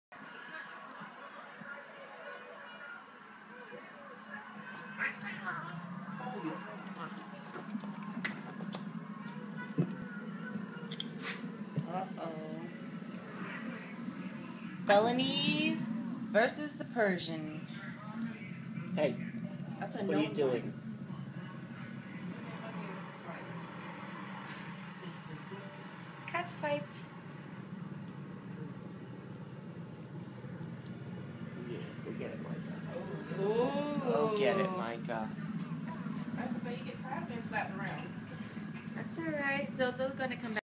Cat fight!
Belongs e VS Persian kitty! Really Micha (Persian) just wanted Zoeys spot!